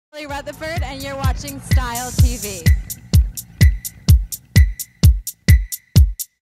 Kelly Rutherford endorses styleTV
Kelly Rutherford most famously known for starring in Gossip Girl endorses styleTV